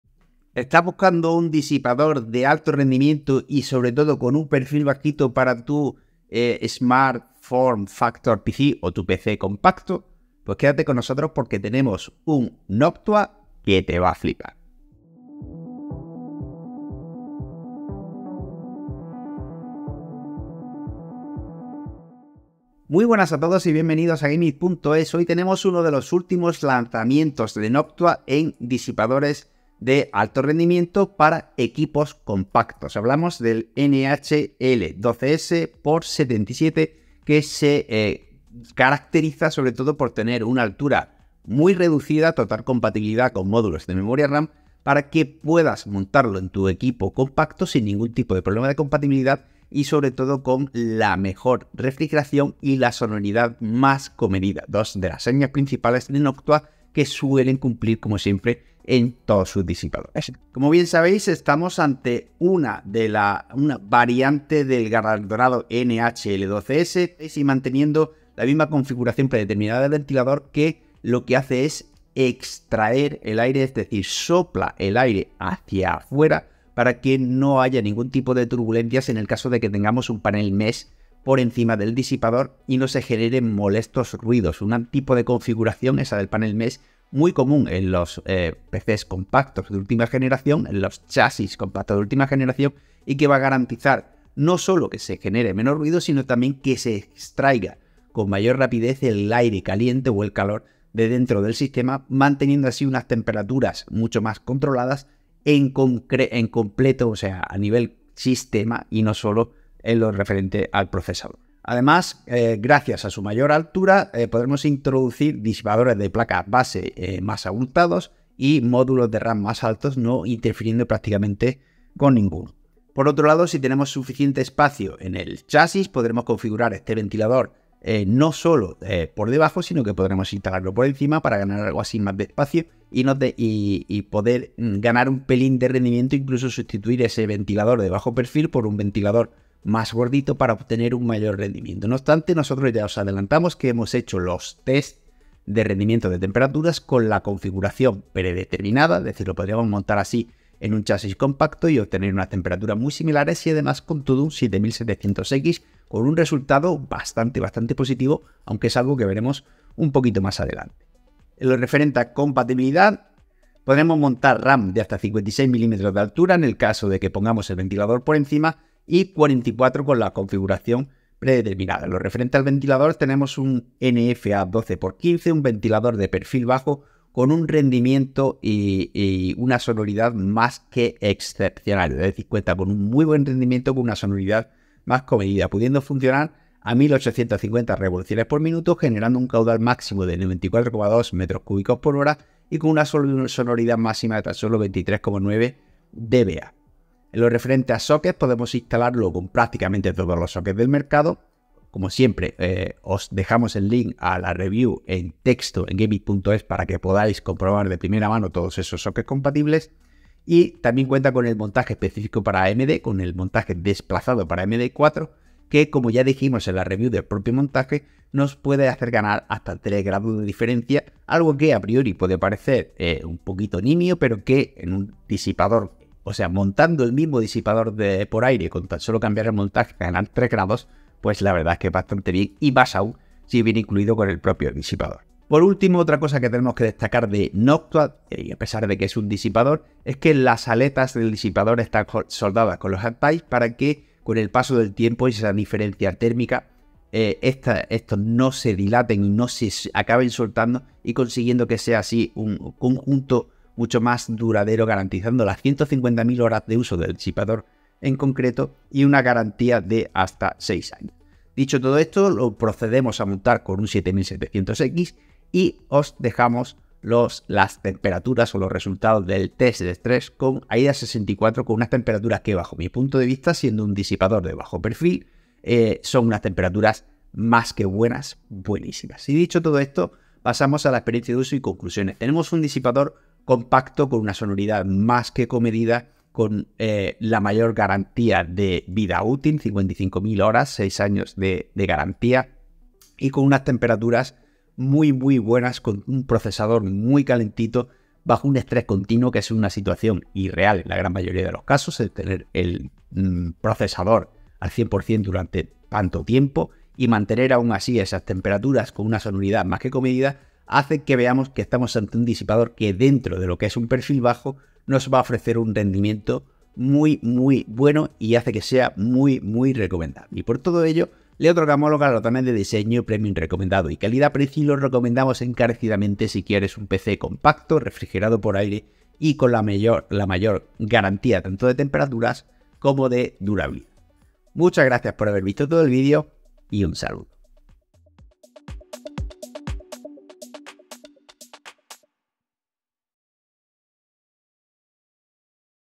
Noctua NH L12Sx77 review del mejor disipador de bajo perfil de Noctua | GameIt ES
Link de compra

Analizamos el Noctua NH-L12Sx77, un disipador de tamaño compacto que ofrece un rendimiento sin precedentes